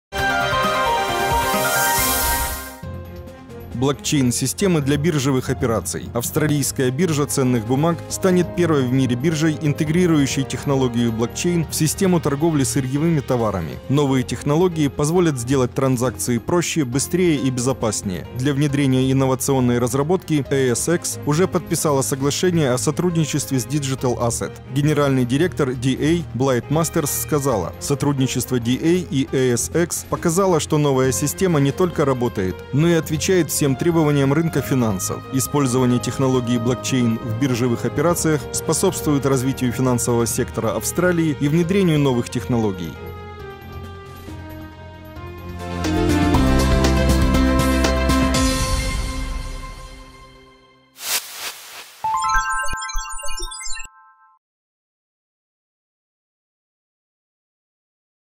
Первая фондовая биржа переходит на блокчейн
Блокчейн-системы для биржевых операций. Австралийская биржа ценных бумаг (ASX) станет первой в мире биржей, интегрирующей технологию блокчейн в систему торговли сырьевыми товарами. Новые технологии позволят сделать транзакции проще, быстрее и безопаснее. Для внедрения инновационной разработки ASX уже подписала соглашение о сотрудничестве с Digital Asset (DA). Генеральный директор DA, Блайт Мастерс, сказала: «Сотрудничество DA и ASX показало, что новая система не только работает, но и отвечает всем требованиям рынка финансов». Использование технологии блокчейн в биржевых операциях способствует развитию финансового сектора Австралии и внедрению новых технологий.